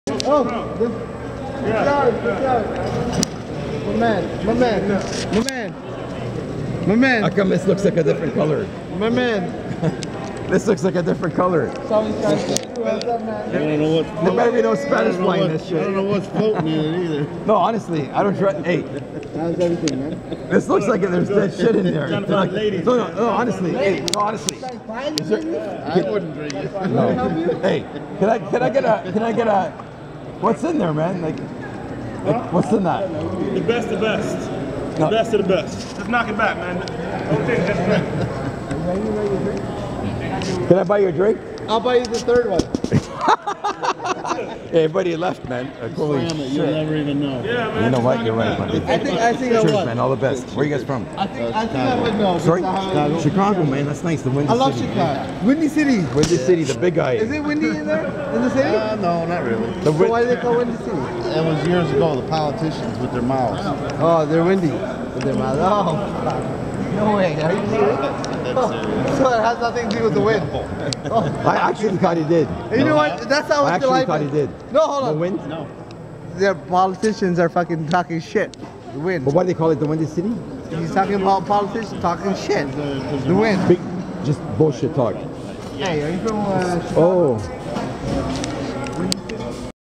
Bahamas inside bar at RIU hotel, talking and fooling around. Feb 2016
Fooling around and talking nonsense in the RIU hotel in Bahamas.